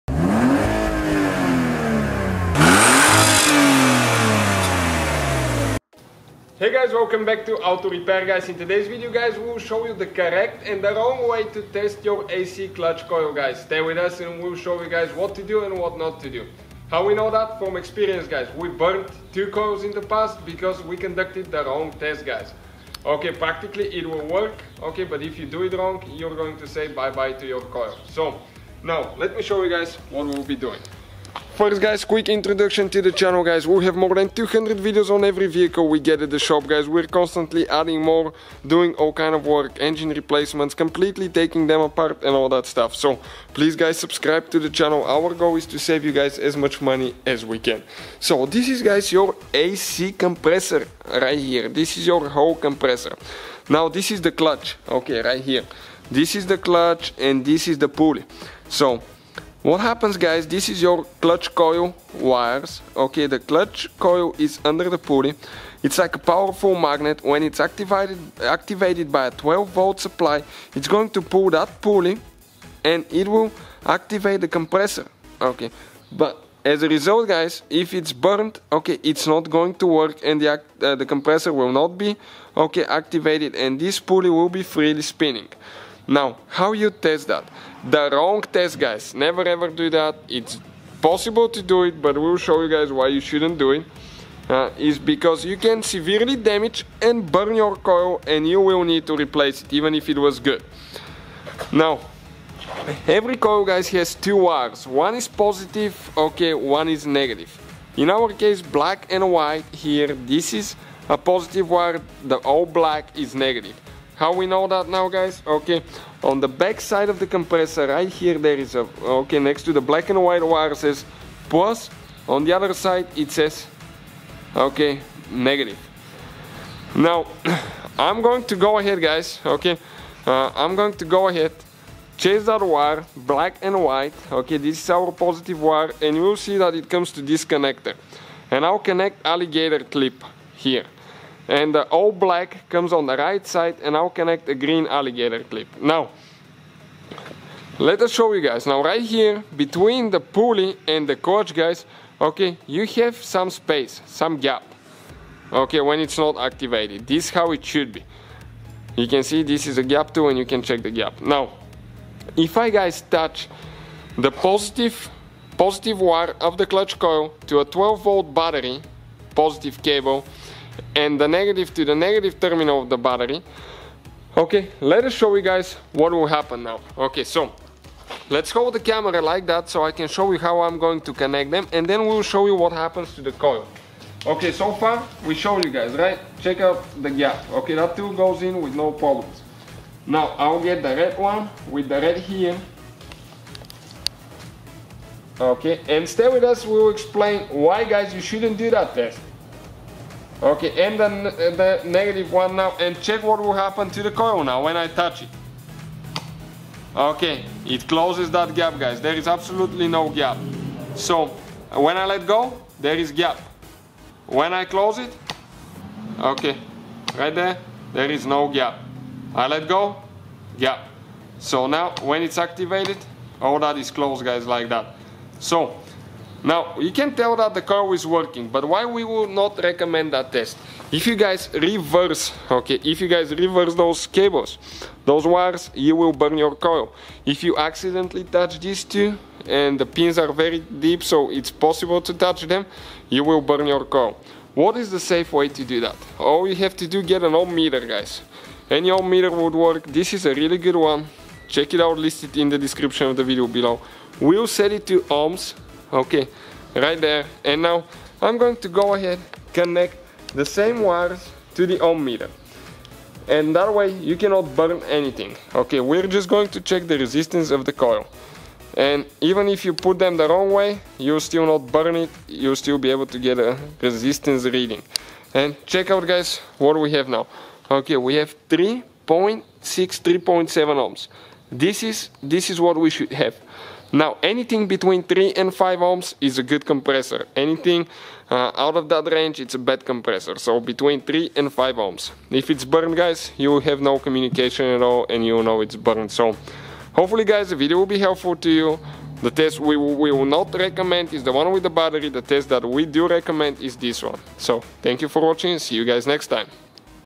HOW TO TEST AC CLUTCH COIL. A/C CLUTCH TEST WHAT NOT TO DO
If your A/C is not working and blows hot air we will show you how to test your air conditioner compressor clutch coil also known as AC Clutch Coil. If the AC Clutch is bad then your air conditioner will not work because the a/c clutch activates the compressor. In this video we will explain the correct way to test AC clutch and how to know if you need to replace your A/C Clutch or the problem is somewhere else. We have a video on the channel that explain why ac may not be working. We will cover everything from start to finish that you need to know about the ac clutch test.

► Voltmeter Voltcraft